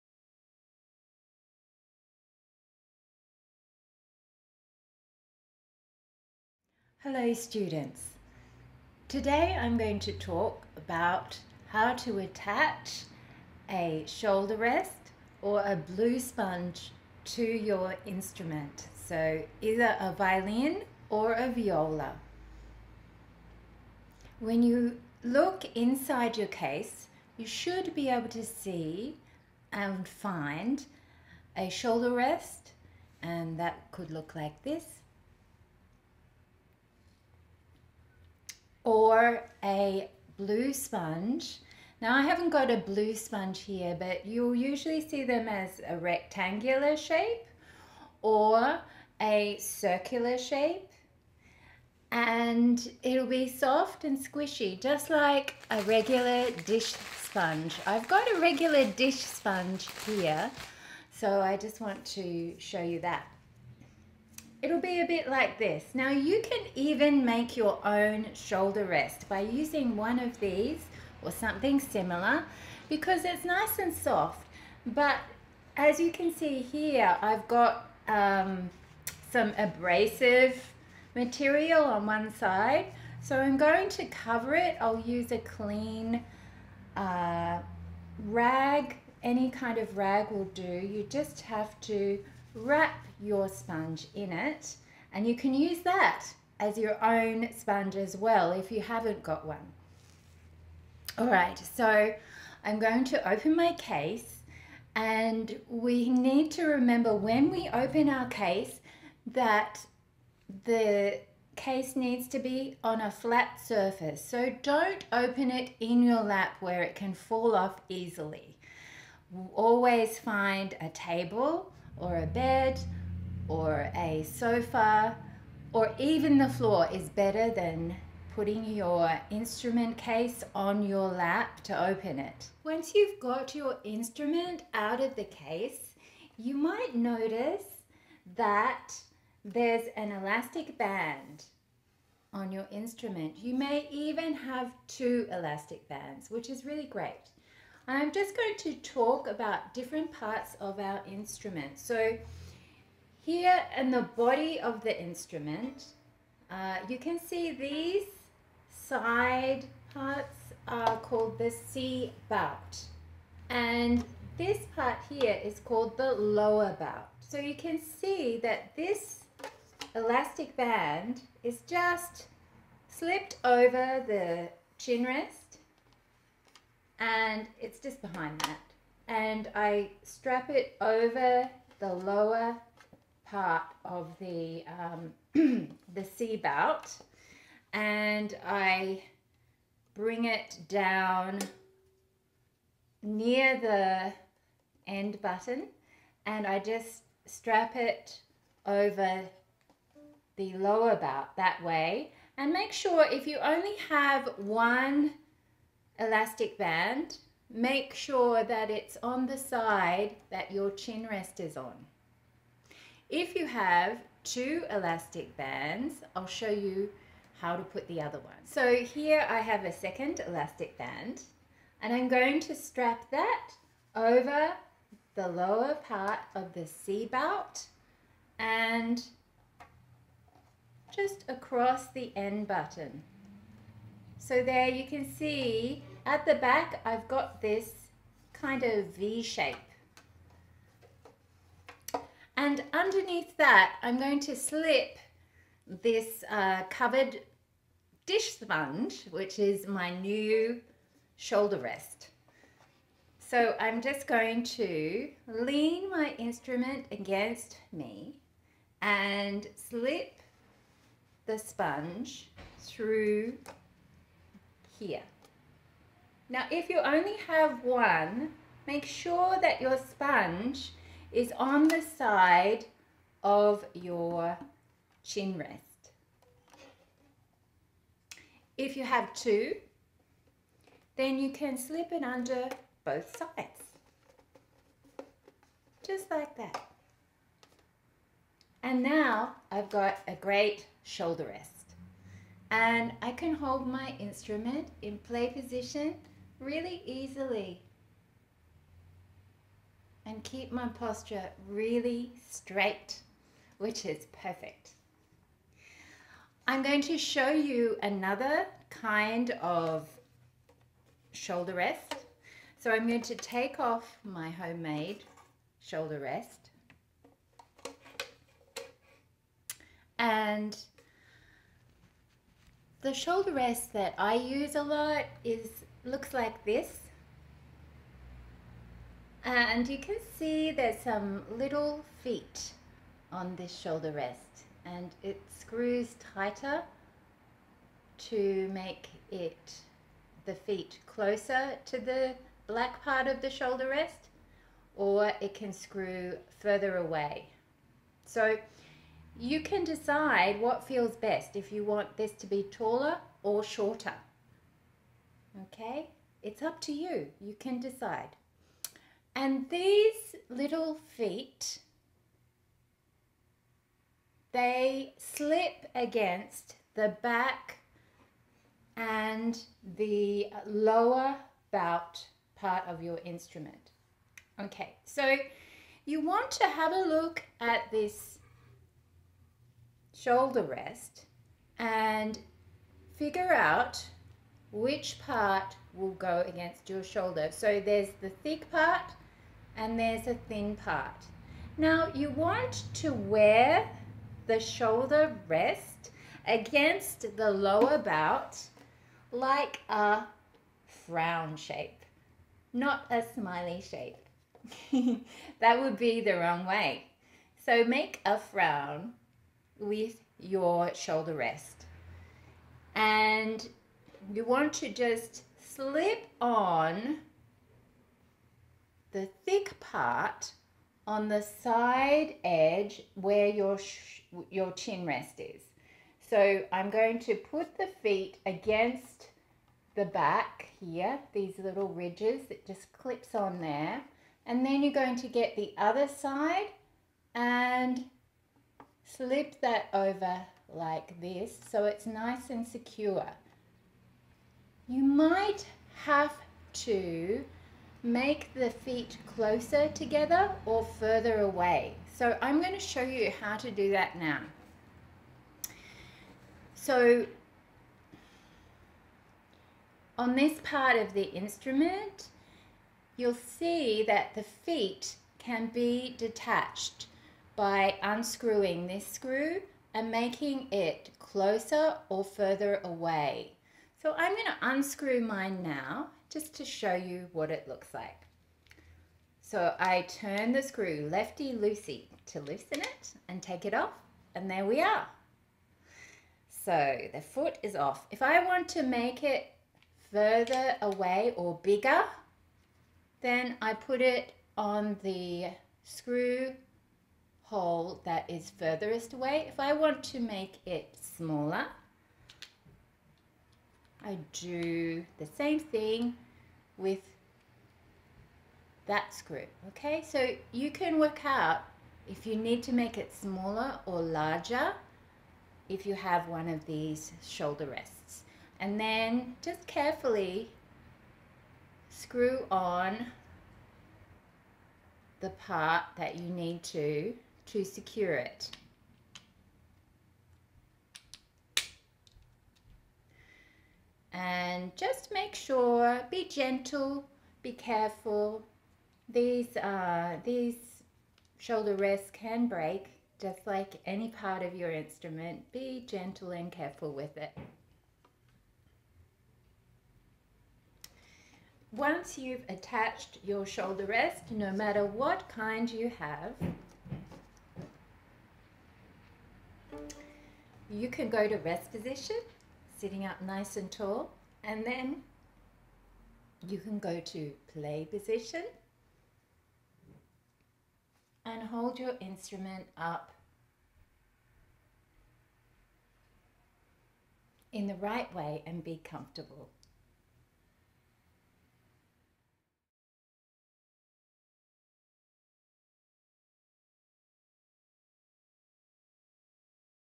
Shoulder Rest for Violin and Viola
How to make your own shoulder rest and attach it properly to your violin or viola